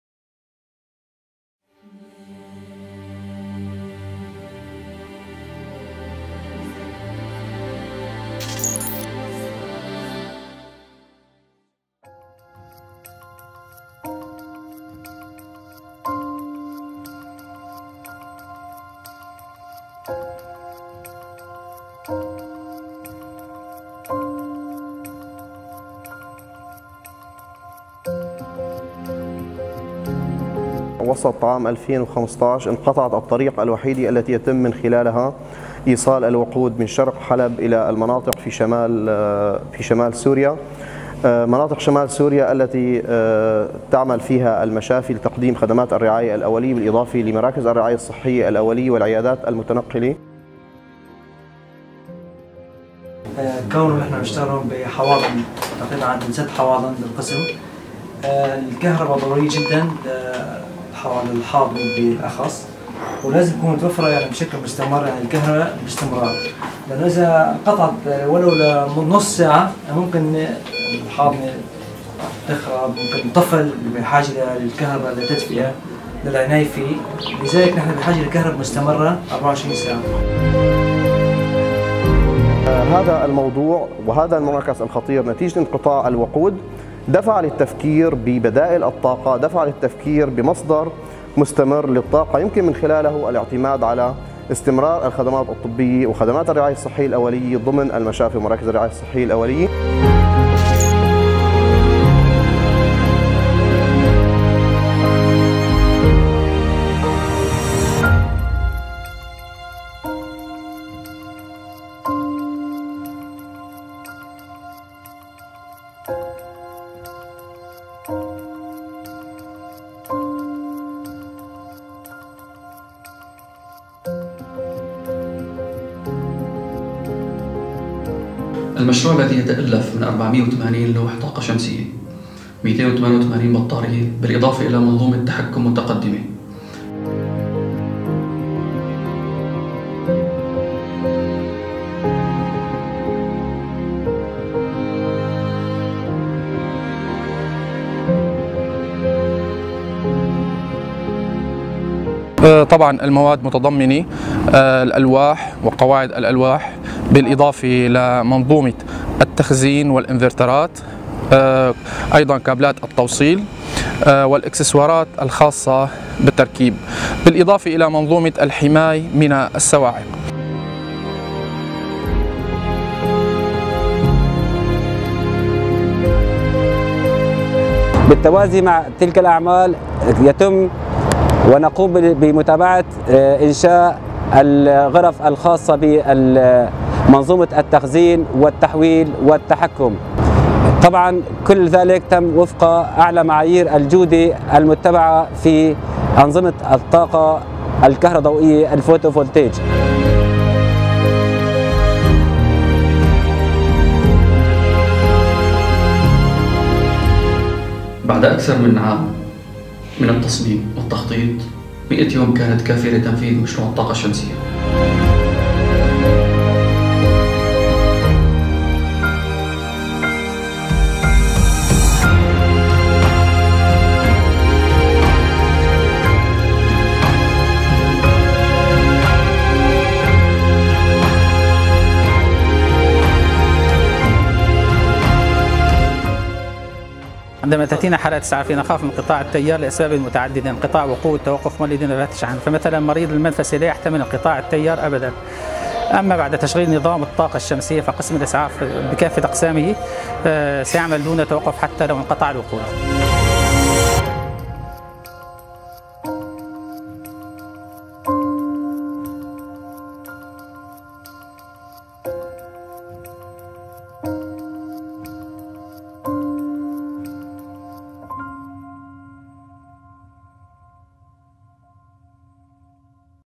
Resilience for Hospitals & Renewable Energy in Syria | UOSSM
Renewable energy can play a big role in strengthening local communities through empowering critical infrastructure with clean-sustainable electricity. In Syria, with our partners, we developed and deployed a system for a critical hospitals in Syria. The solar photovoltaic systems includes 480 solar panels, advanced controls and an energy storage system. The diesel costs will be reduced significantly, and life-savings sections will operate all the time. Solar energy is a democratizing force and can bring power to the people.

- Syria Solar is an initiative by UOSSM to transition health facilities in Syria from diesel dependency to renewable energy
- UOSSM is an international network of medical organizations working towards supporting the medical system in Syria
لدى الطاقة المتجددة امكانيات ضخمة لتمكين المجتمعات المحلية من خلال تقوية البنى التحتية الحساسة وتشغيلها بطاقة نظيفة ومستدامة كبديل للوقود والديزل.. عملنا مع شركائنا في سوريا على تطوير وتنفيذ منظومة طاقة شمسية رائدة لأحد أهم المشافي في سوريا. المنظومة تتضمن أكثر من ٤٨٠ لوح طاقة شمسية بالإضافة إلى نظام تحكم وتخزين للطاقة متطور. من المتوقع أن تنخفض تكاليف التشغيل و المازوت بشكل كبير والأهم أن يستمر عمل الأقسام الحساسة بشكل متواصل حتى في حال انقطع امداد المازوت عن المنطقة.

شمس سوريا هي مبادرة من اتحاد منظمات الرعاية الطبية والاغاثية لتمكين المنشآت الطبية في سوريا بالطاقة المستدامة والمتجددة.